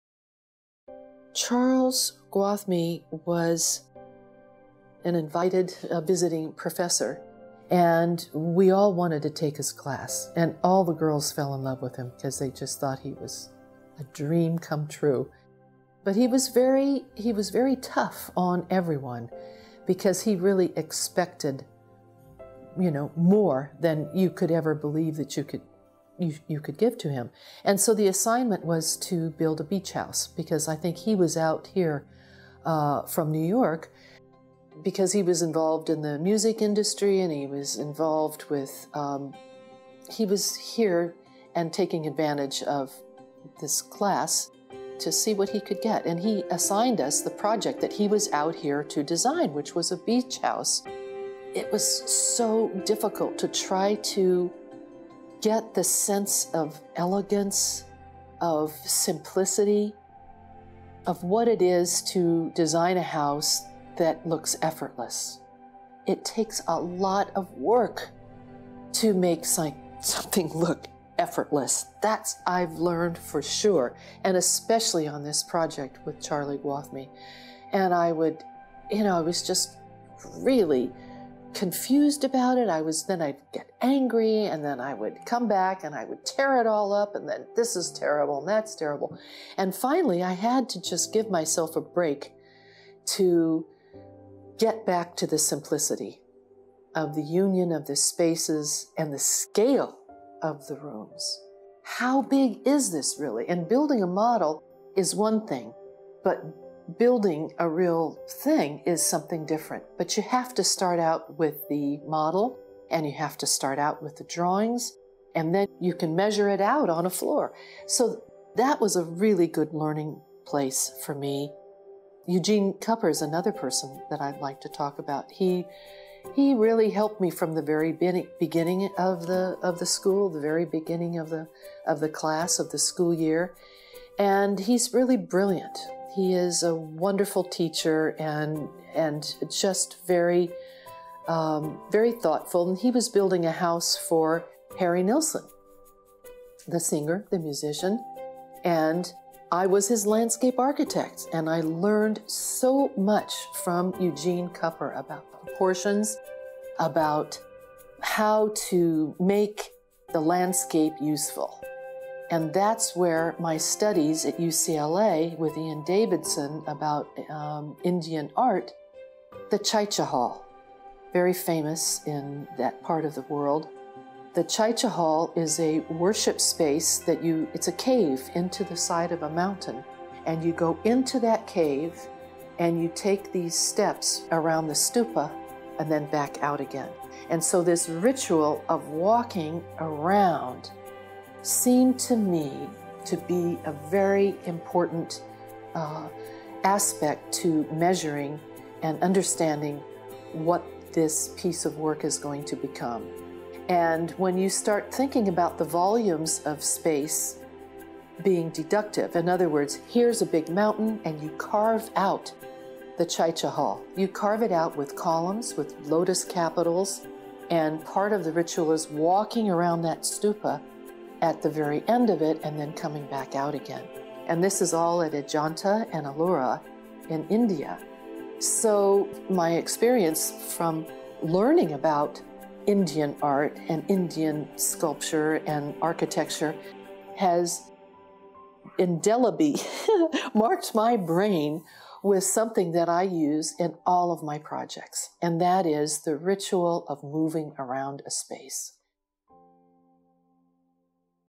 Pamela Burton on Design: Mentors [1 of 10]
Architect Charles Gwathmey’s teachings explored the union of spaces and the scale of room. Working on the Nilsson residence with architect Eugene Kupper, Burton experimented with how to make the landscape useful. Ian Davis’ course on Indian art included the ritual of walking.